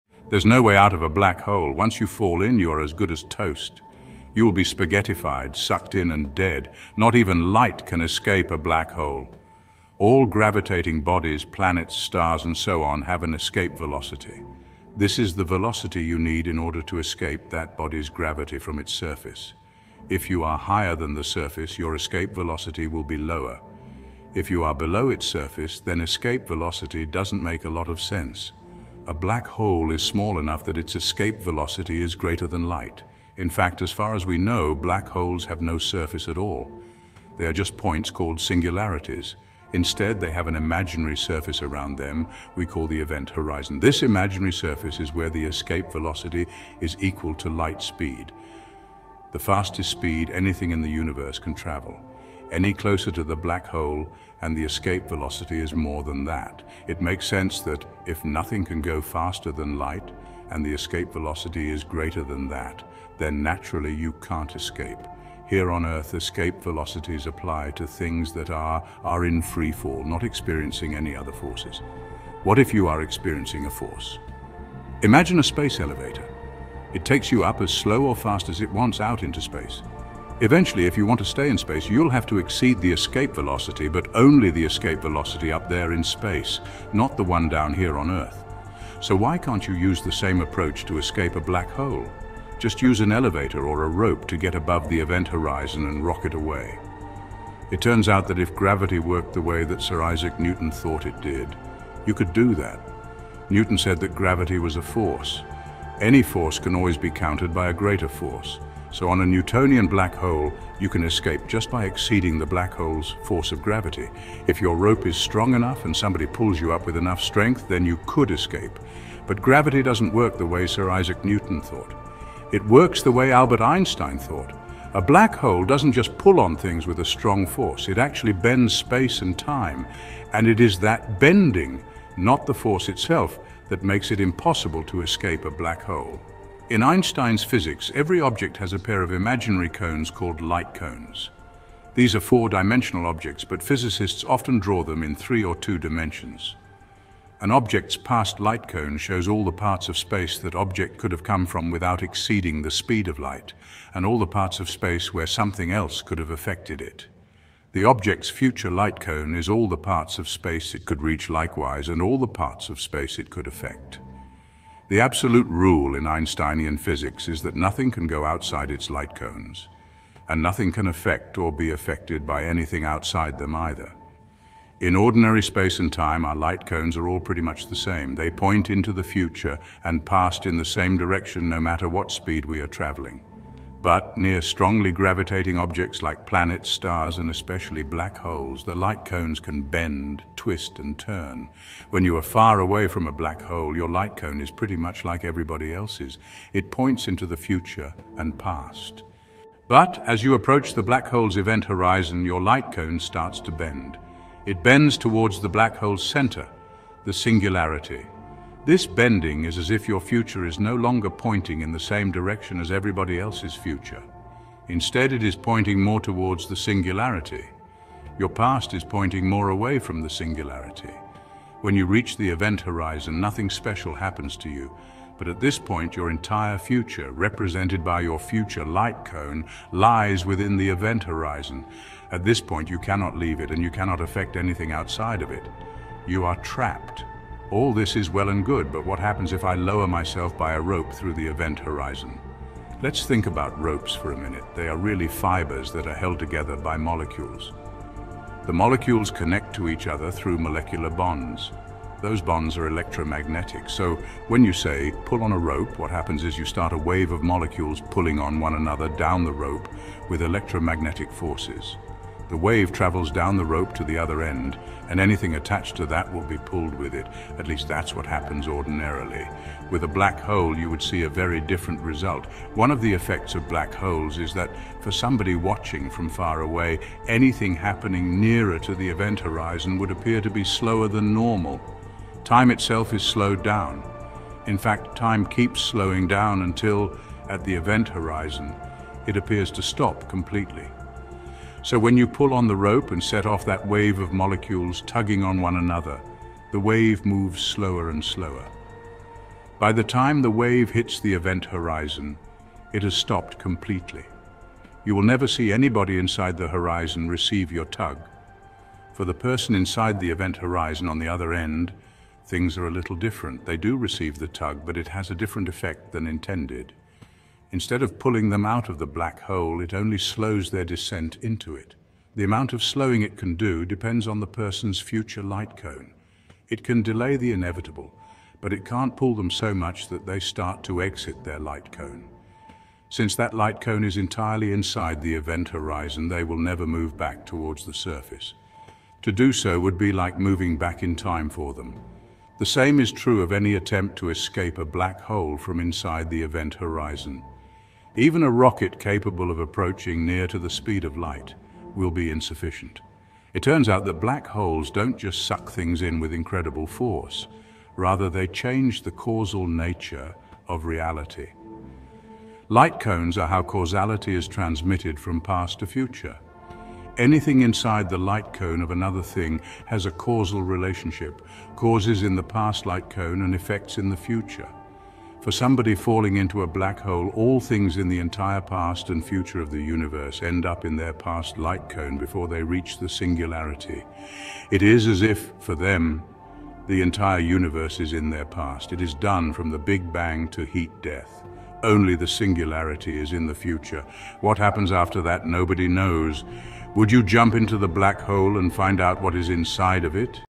Can You Use A Rope To Rescue A Person From A Black Hole? Using Newtonian & Einstein 's Physics - YES
The video explains the concept of black holes, focusing on why escape is impossible due to the bending of space and time rather than just gravitational force.

At the core of the explanation is the idea of escape velocity, which is the speed needed to escape a gravitational field. For black holes, the escape velocity exceeds the speed of light, which means even light cannot escape. This creates the event horizon, the boundary beyond which nothing can return.

If gravity worked as Sir Isaac Newton theorized, it would be a force that could be countered by another force. In that case, a strong enough pull or push (like from a rope or rocket) might allow someone to escape a black hole. However, gravity, as described by Albert Einstein, bends spacetime itself. This curvature causes lightcones—mathematical representations of an object’s causal future and past—to bend toward the black hole's singularity as an object gets closer.

Near the event horizon, an object’s future lightcone points entirely into the black hole, meaning its future trajectory is locked inside. Even if an external force (like a rope) could slow an object’s descent into the black hole, it cannot reverse time or push the object out.

Thus, black holes don’t just exert strong forces but also alter the fabric of spacetime, preventing escape once inside. At the singularity, causal relationships between past and future break down, and it remains a mystery what happens beyond that point.